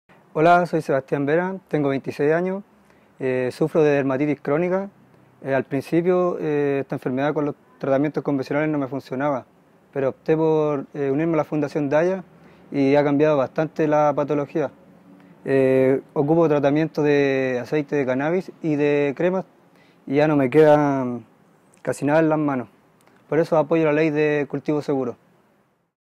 Ley cultivo seguro: Sebastián
Sebastián trata su Dermatitis crónica con cannabis medicinal. Como el, hay miles de pacientes que encuentran alivio con cannabis. Necesitamos LeyCultivoSeguro para proteger el derecho a cultivar sin miedo de todos y todas. Por la salud, seguridad y dignidad